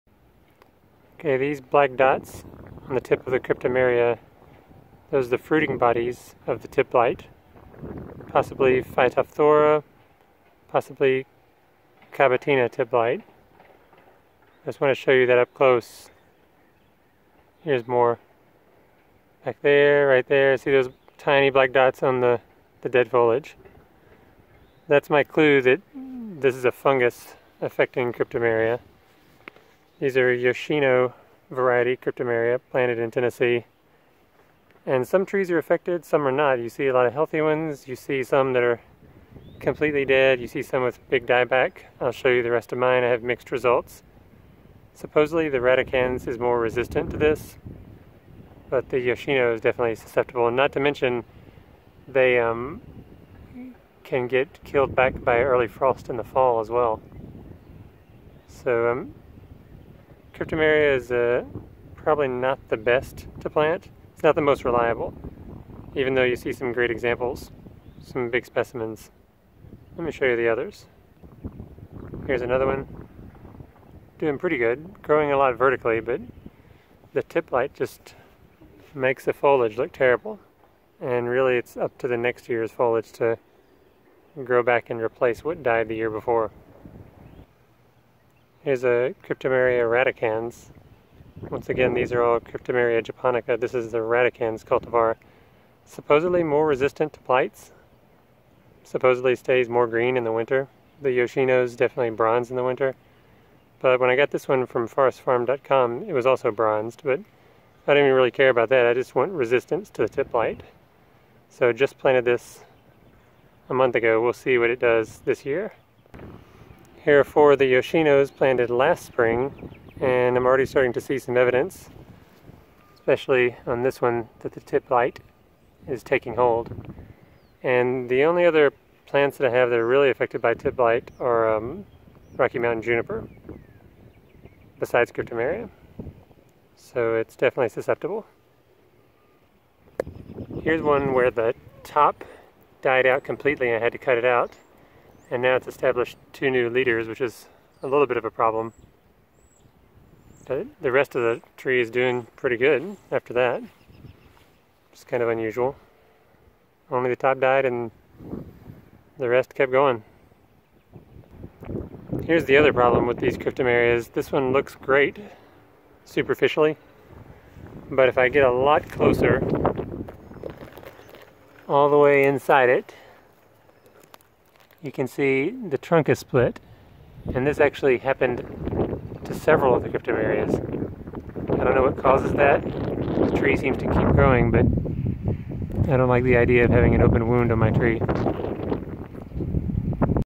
Cryptomeria (Japanese Cedar) planted in middle Tennessee affected by tip blight
CORRECTION: The Radicans is from fastgrowingtrees.com, not forestfarm.com.  These examples of cryptomeria japonica show how the tree is affected by tip blight and other problems.  Most likely phomopsis or kabatina tip blight, or both working together.